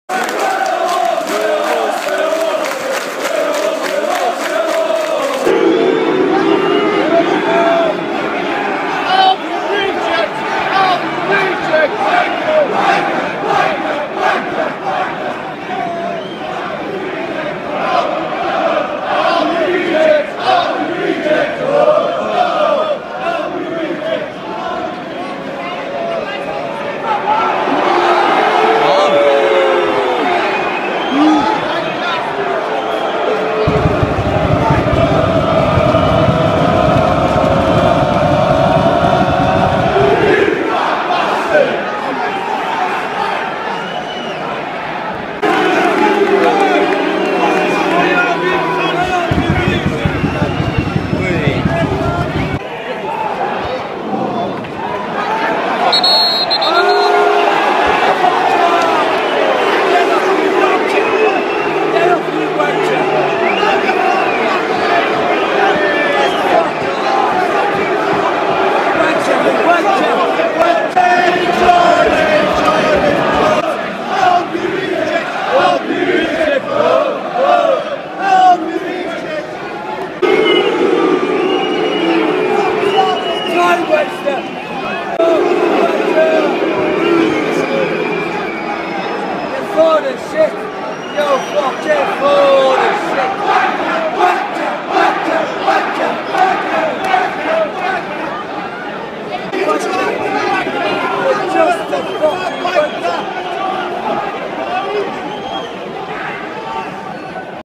Wolves fans taunt Scott Carson vs Derby (27/2/16)
Ex West Brom Goalkeeper Scott Carson had a tough time, getting taunted by the Southbank.